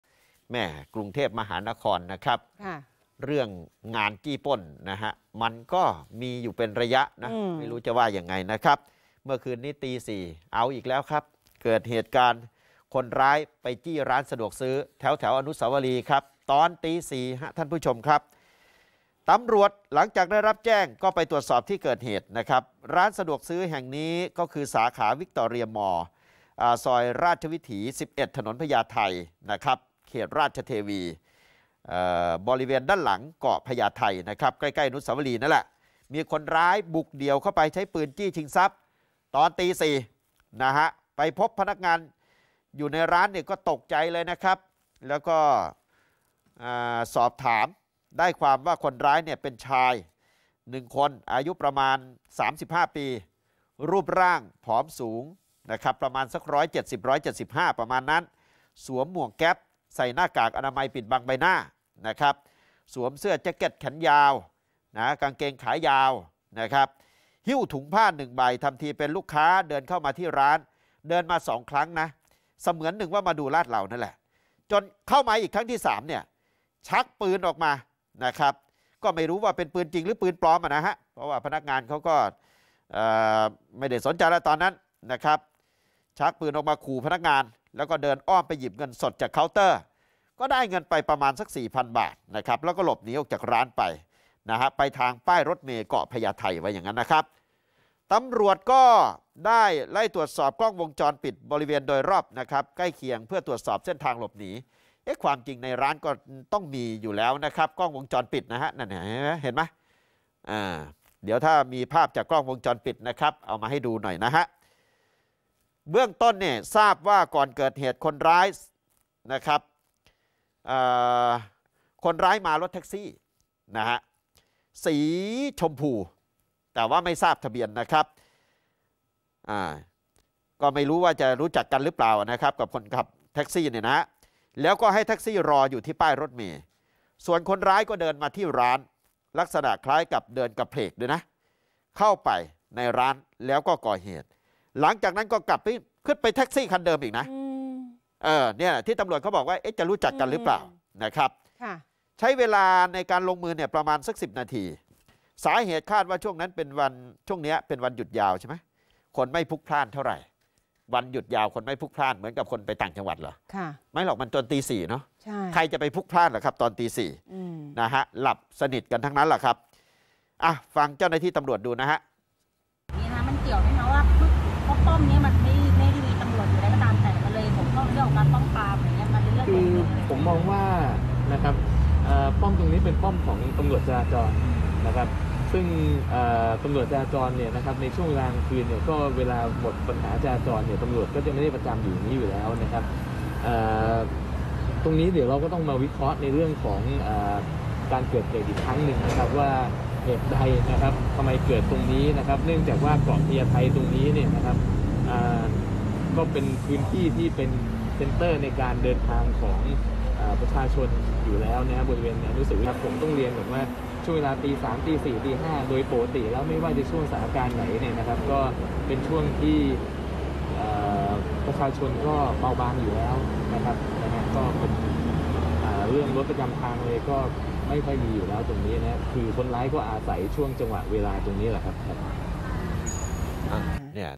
บุกเดี่ยวจี้ร้านสะดวกซื้อกลางอนุสาวรีย์ฯ กวาดเงินไป 4,000 บาท
อุกอาจ! โจรบุกเดี่ยวใช้อาวุธปืนจี้ร้านสะดวกซื้อย่านอนุสาวรีย์ชัยฯ กวาดเงินไป 4,000 บาท ขึ้นแท็กซี่ที่จอดรอหลบหนี